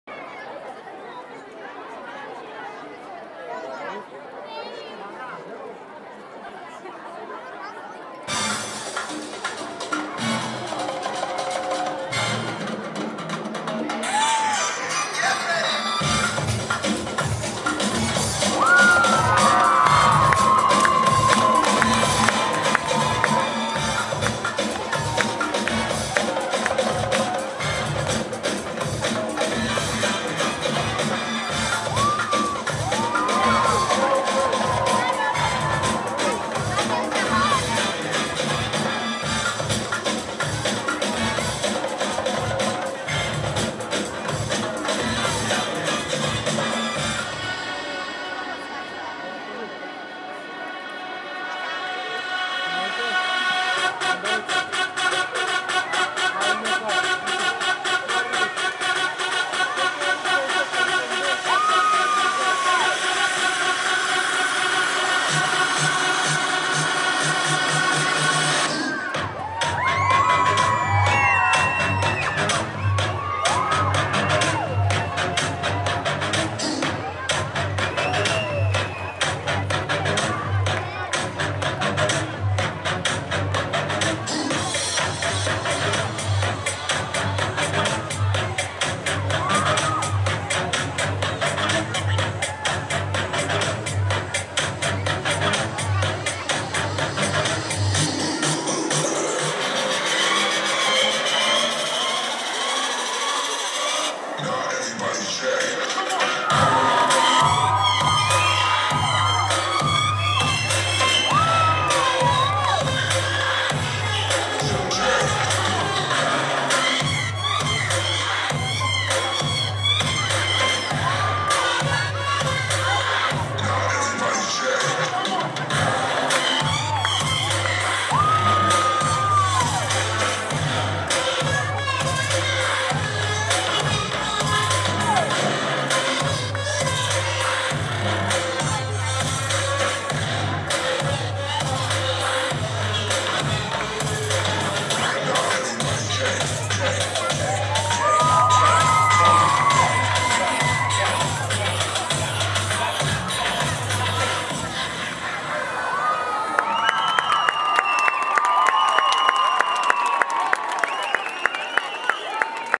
Fantasy kids
X s Dance Studio- Fantasy Kids ,Locul 1 , Freestyle 4-7 ani , Dance art Open 2016 Navodari